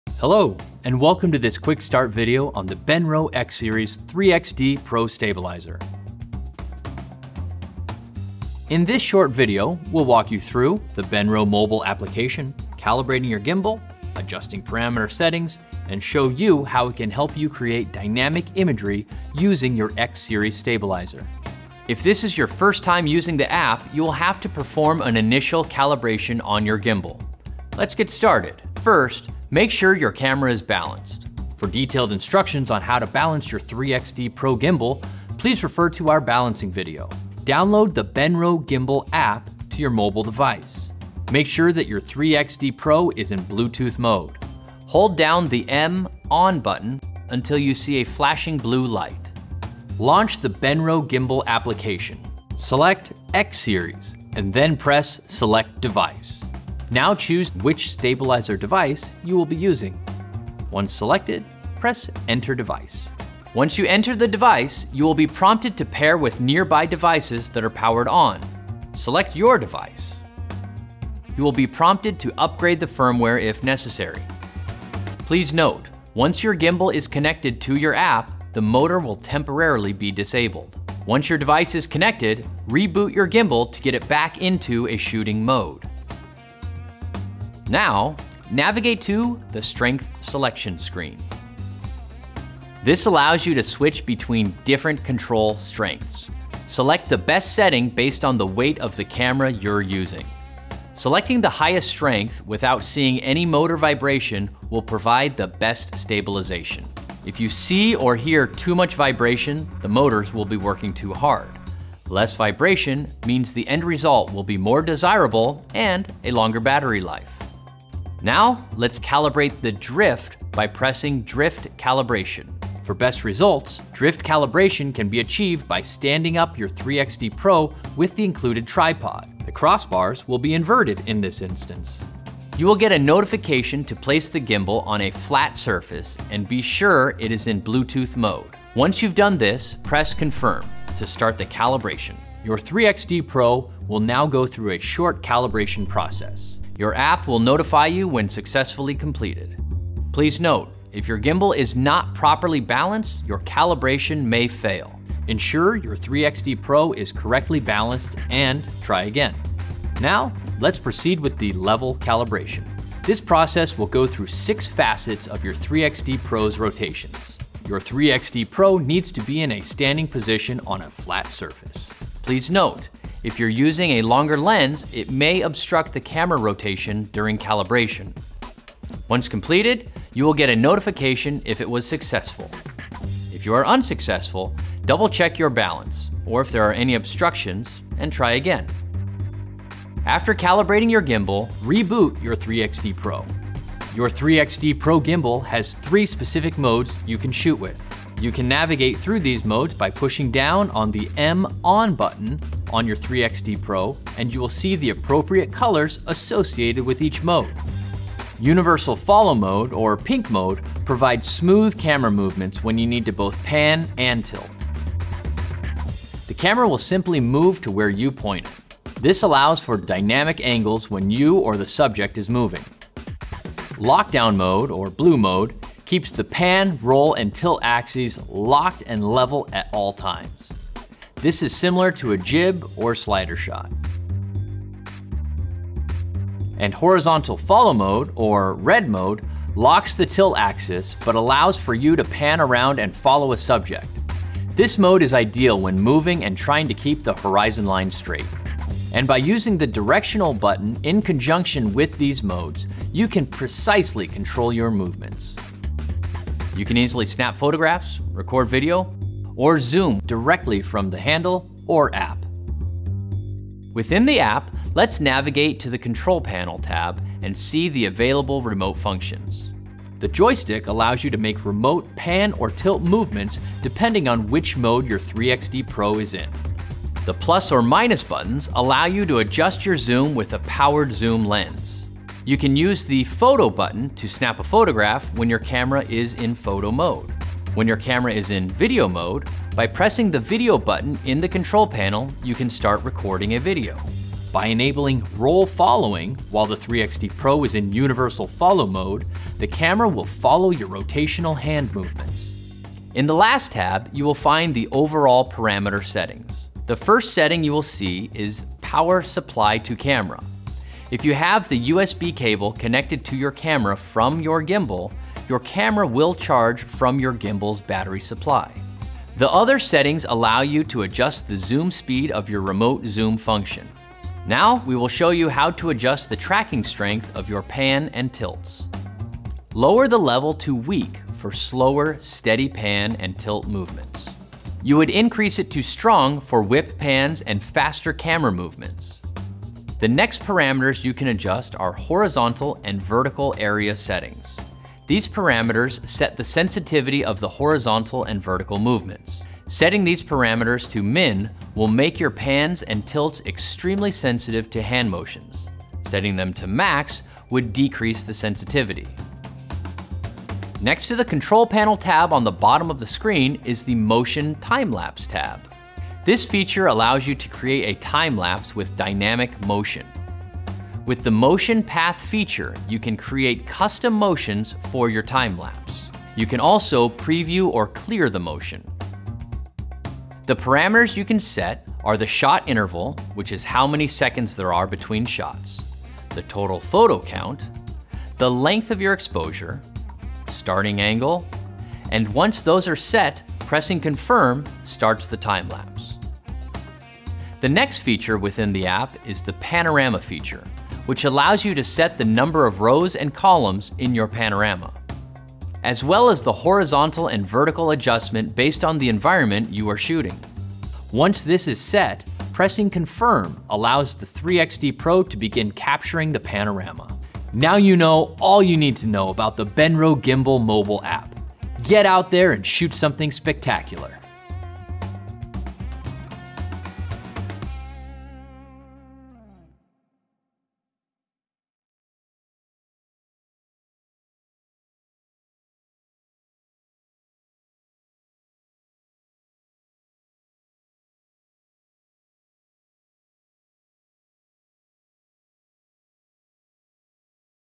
Benro X-Series 3XD Pro Quick Start - App Walkthrough
In this video we’ll walk you through the Benro mobile application, calibrating your 3XD Pro Gimbal,  adjusting parameter settings, and show you how it can help you create dynamic imagery using your X-Series stabilizer.